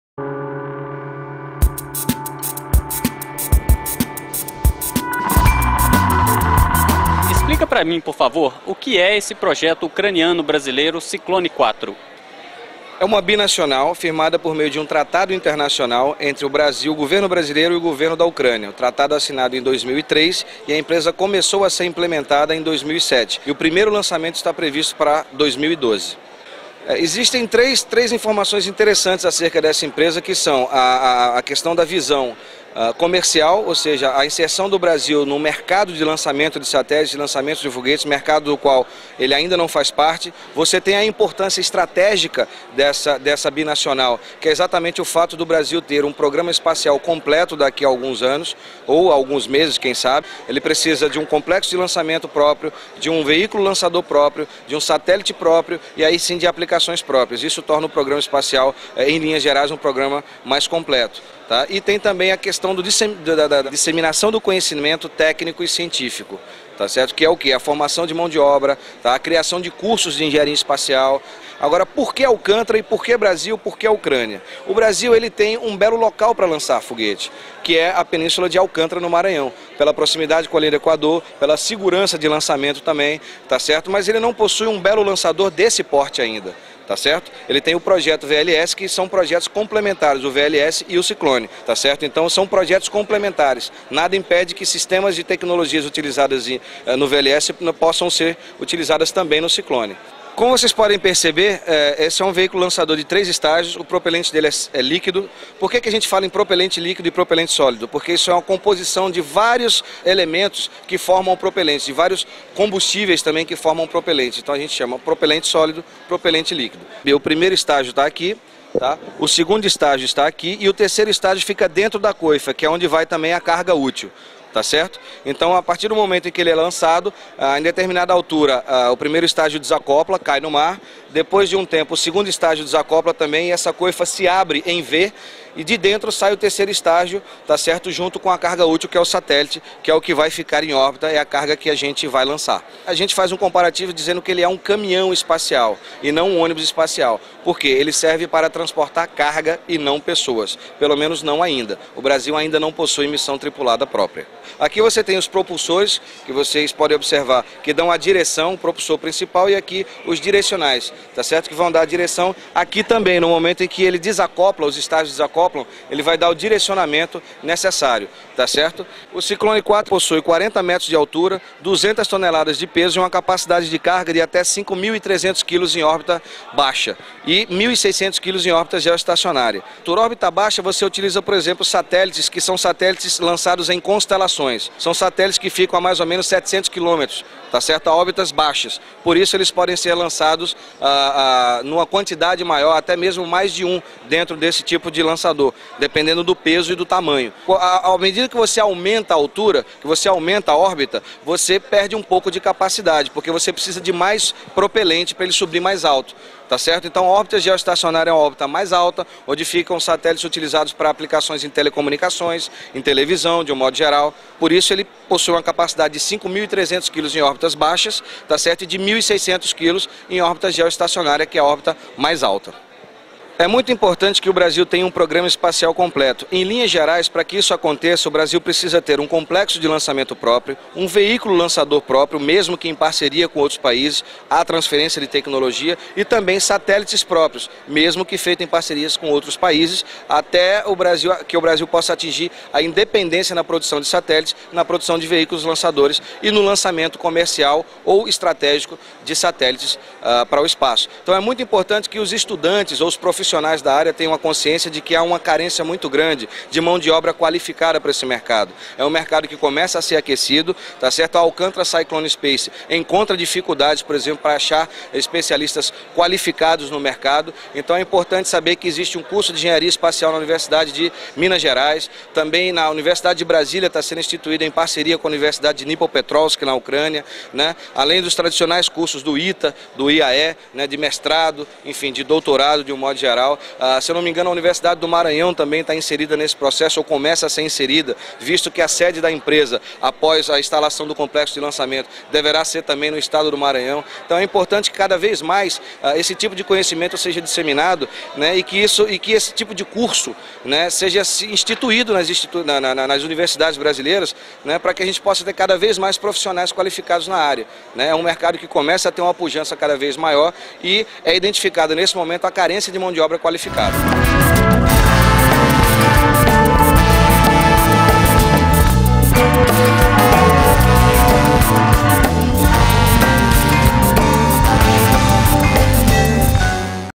Projeto do Lançador Cyclone-4 - Entrevista ao Portal Pontociência
Vídeo de reportagem realizada pelo "Portal Pontociência" durante a feira tecnológica da "62ª Reunião Anual da Sociedade Brasileira para o Progresso da Ciência (SBPC)" ocorrida de 25 a 30/07/2010 em Natal (RN), sobre o projeto do foguete lançador de satélites ucraniano Cyclone-4 da mal engenhada empresa bi-nacional "Alcântara Cyclone Space. O vídeo só foi editado e postado no YouTube em abril de 2011.

No vídeo, o representante da ACS, André Barreto, que é entrevistado pelo repórter Leandro Fantini, fala sobre o acordo Brasil e Ucrânia e algumas das especificações técnicas desse foguete ucraniano.

O Portal Ponticência é uma realização da Universidade Federal de Minas Gerais (UFMG) com o apoio da FINEP/MCT e tem como parceira a "Secretaria de Ciência e Tecnologia para a Inclusão Social (SECIS).